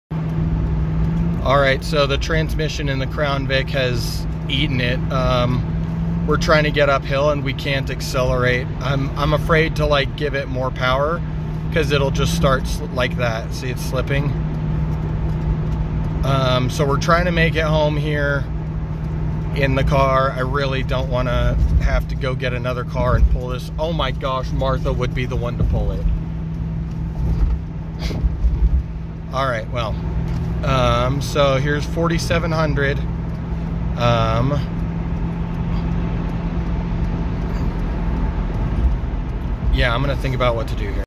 So my Crown Vic died
losing the transmission in the crown vic on the way home..